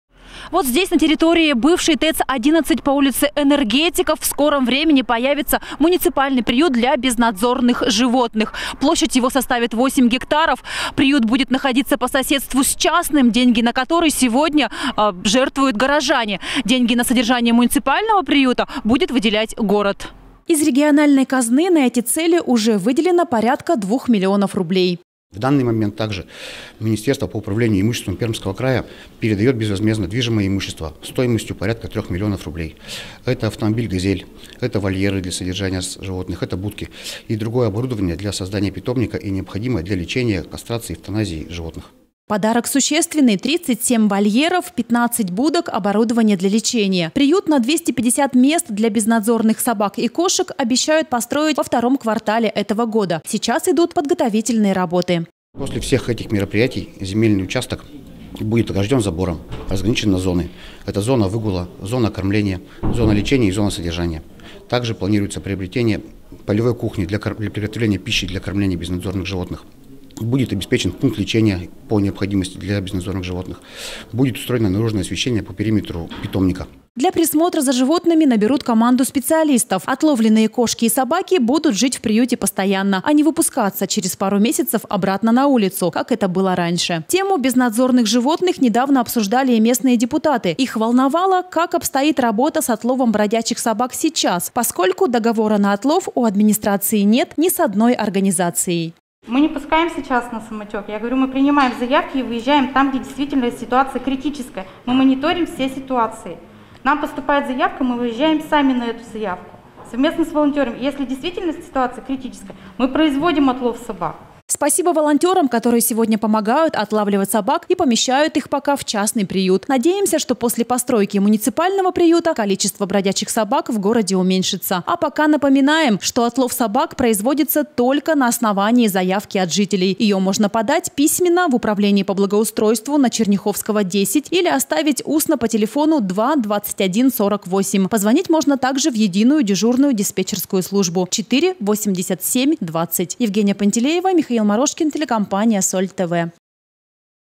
Приюту для животных быть_24.03.2021_СольТВ
Собачья тема сегодня особенно актуальна. Весной животные играют свадьбы, сбиваются  в стаи и становятся агрессивнее.  Приют для безнадзорных животных, о котором столько времени говорилось – наконец-то откроется в нашем городе. Кто выделил деньги, и где появится питомник – выясняла наша съемочная группа.